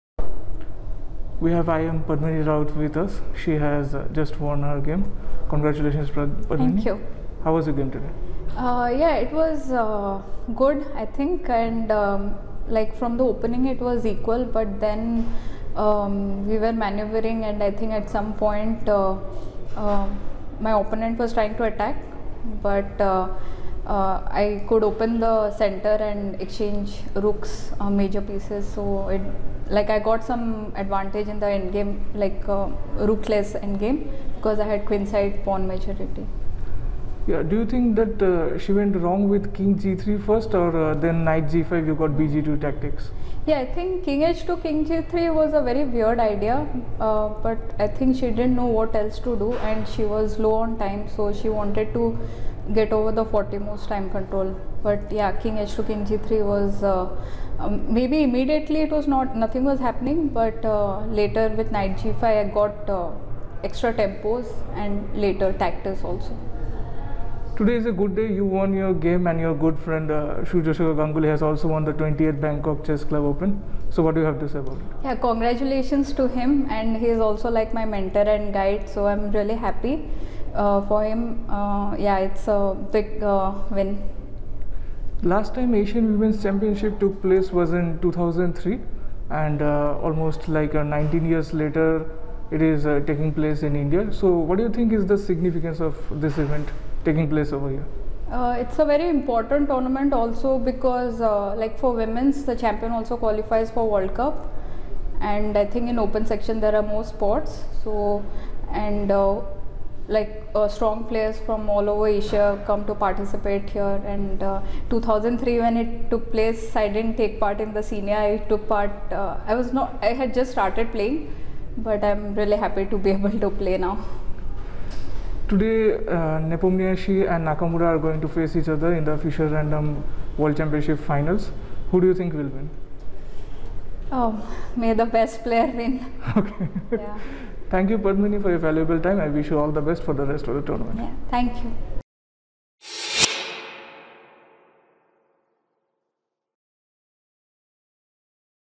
"I am proud that my coach and mentor Surya won in Bangkok" - Padmini Rout
Padmini Rout is well-known exponent of the Grunfeld Defense. In the 5th round of the Asian Continental 2022 she won her game against Kiya Kurmangaliyeva (2199). In this video she tells us about some of the important facets of the Grunfeld opening and how having a queenside majority means that Black is not afraid to exchange pieces. Padmini also talks about her mentor Surya Ganguly winning the tournament in Bangkok. Currently Padmini is on 3.5/5 at the Asian Continental 2022, one point behind the leader.